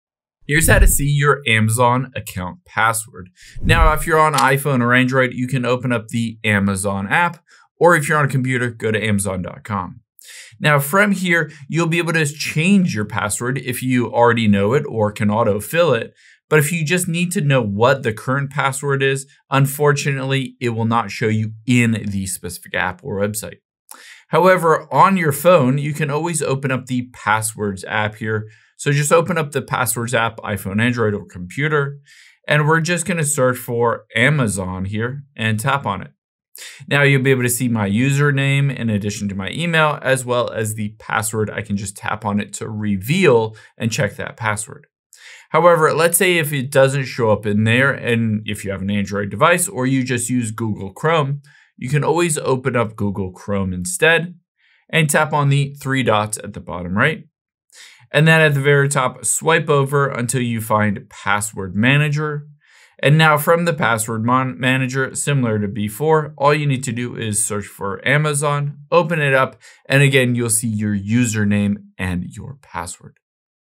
How To See Amazon Account Password
Here's how to see your Amazon account password on your phone or computer if you forgot it.